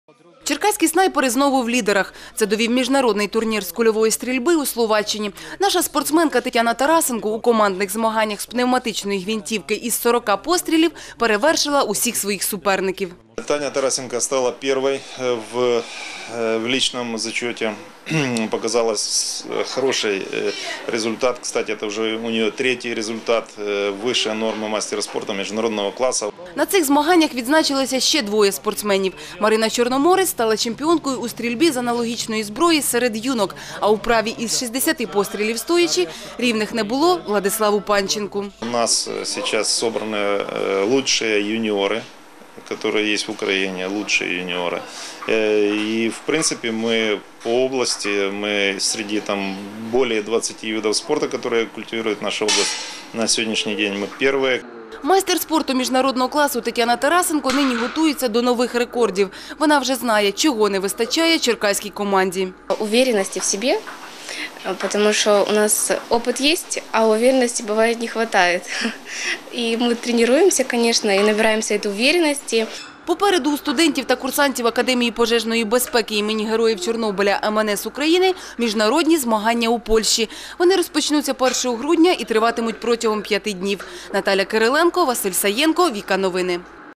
Побили рекорд
Черкащанка побила рекорд украині по пулевой стрельбе